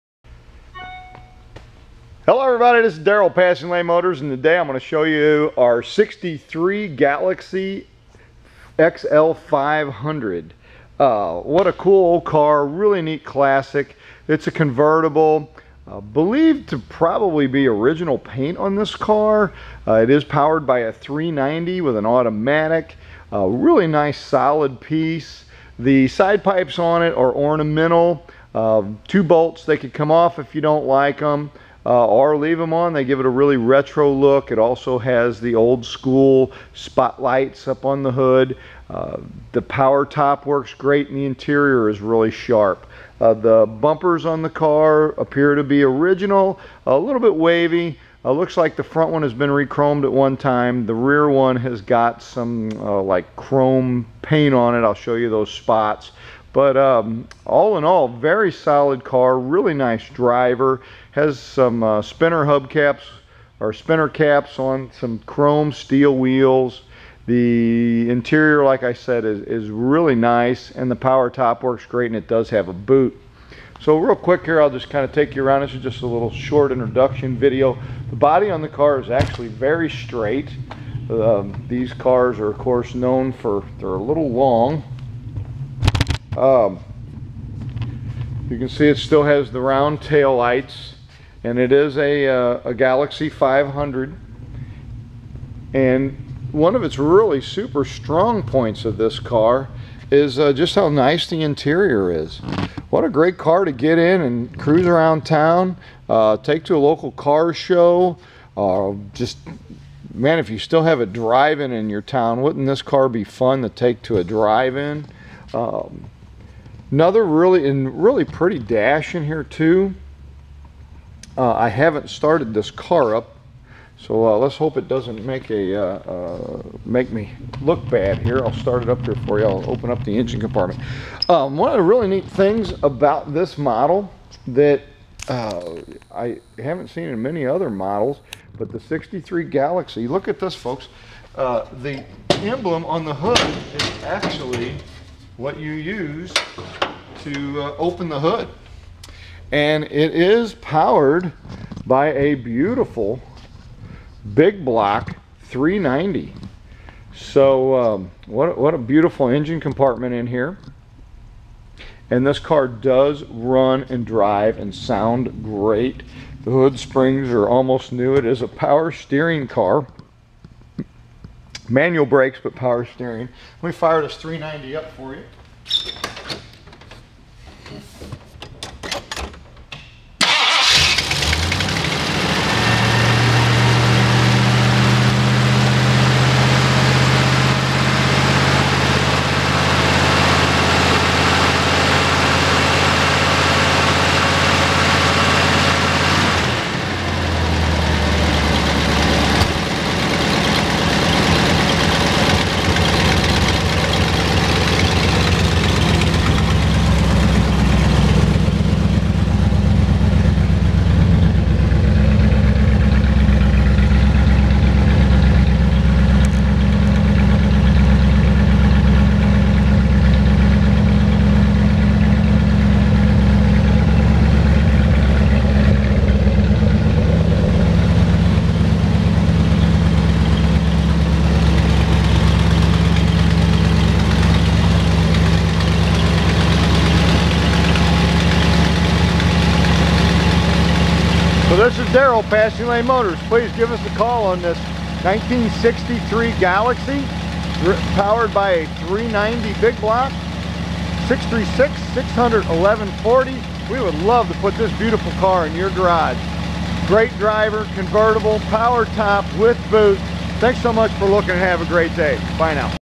***SOLD*** 1963 Gallaxy 500 Convertible, 390, Auto, Clean interior
1963 Gallaxy 500 Convertible, 390, Auto, Clean interior